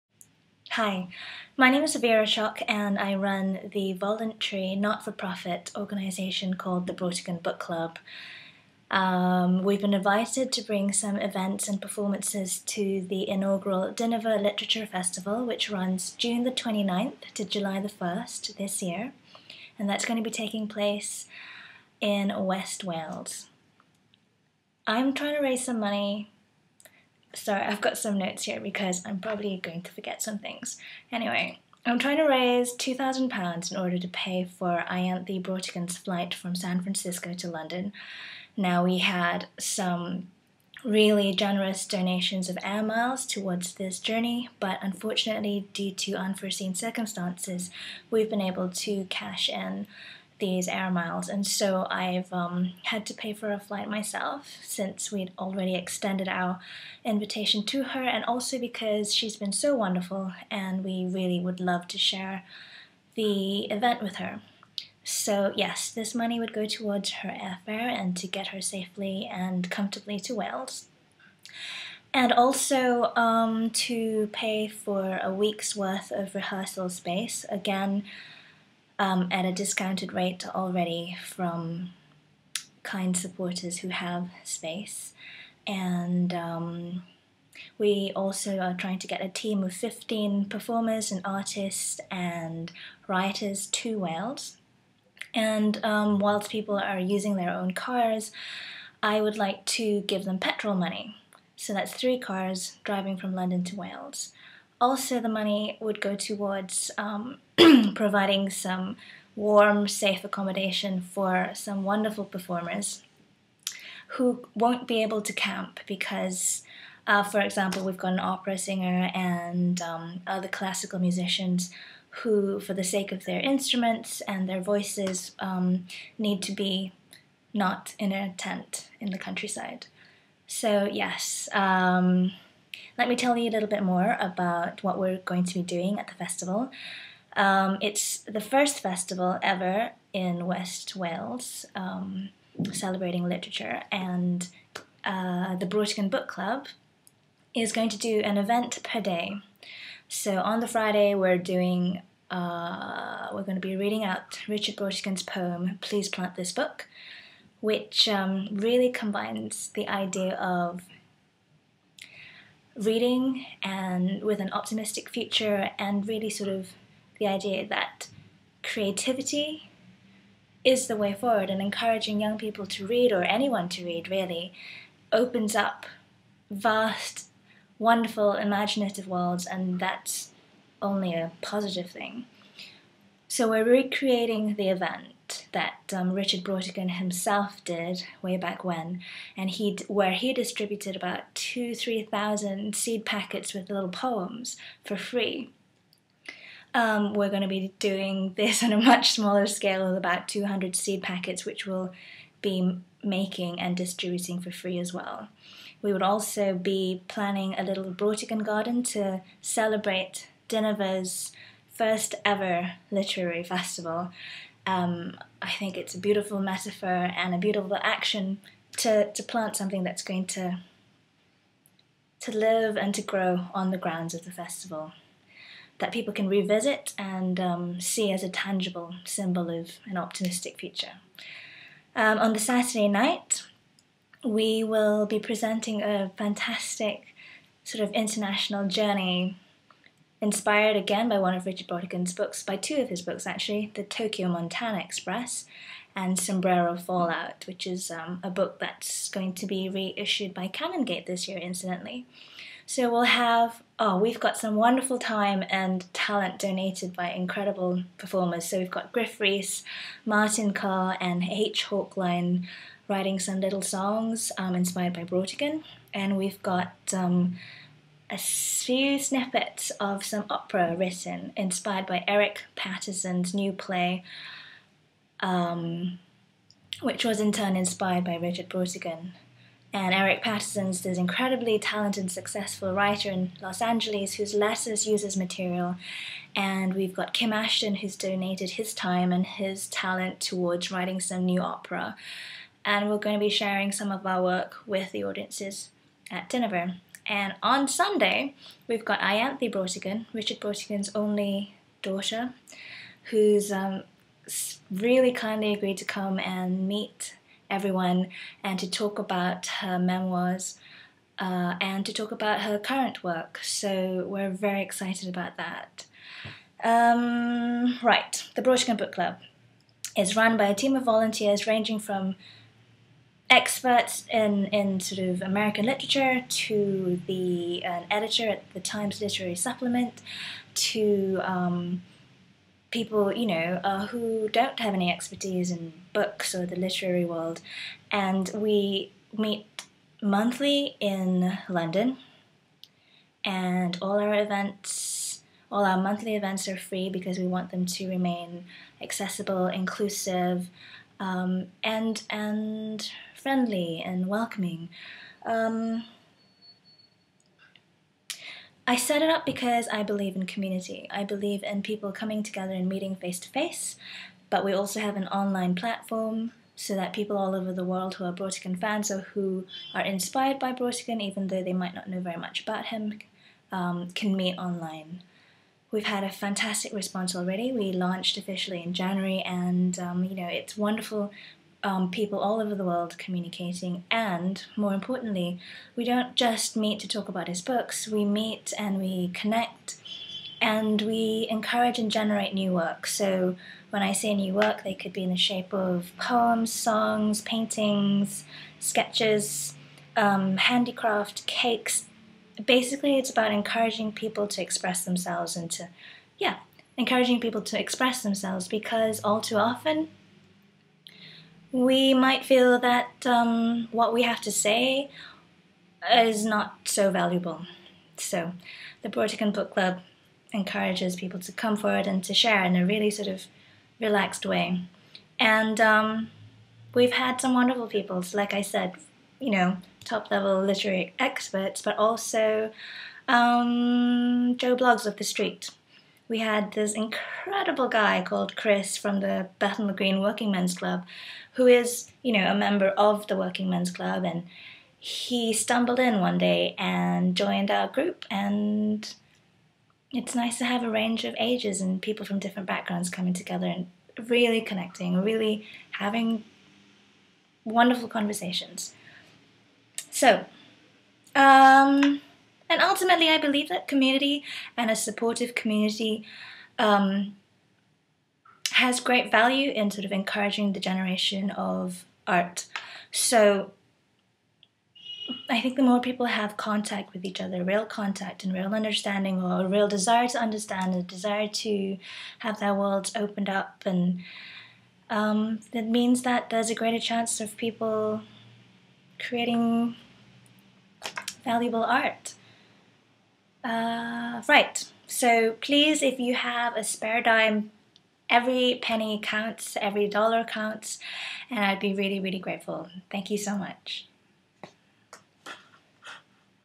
Brautigan Book Club goes to Wales
Fundraising appeal to bring a three-day trilogy of Brautigan events, including a visit from Ianthe Brautigan, to the Dinefwr Literature Festival, Wales, June 29 - Jul 1. Details of our events at Dinefwr are